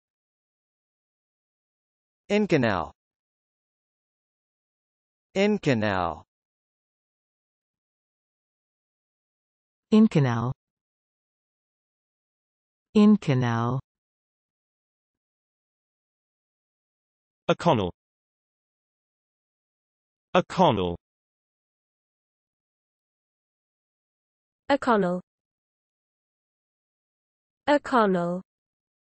How to pronounce inconel
Inconel definitions:
    (n): a nickel-base alloy with chromium and iron; used in gas-turbine blades